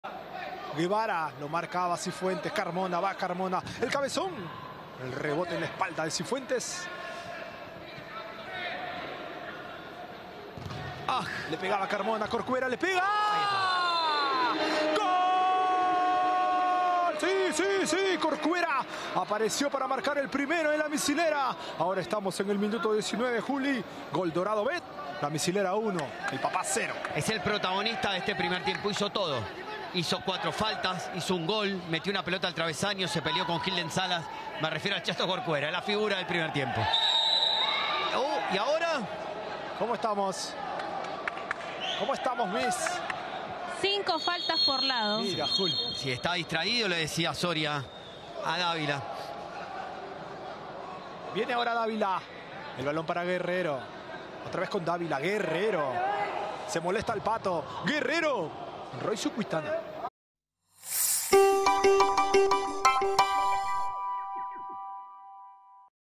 Copa Leyendas de Fútbol 7: José Corcuera anotó así en La Misilera 1-0 El Papá
¡EL PRIMERO DEL PARTIDO! Con un derechazo imposible de detener 🤯, José Corcuera 💥 abrió el marcador a favor de La Misilera ⚽ por 1-0 ante El Papá 🔴, en cotejo por la fecha 6 de la Copa Leyendas de Fútbol 7 🏆.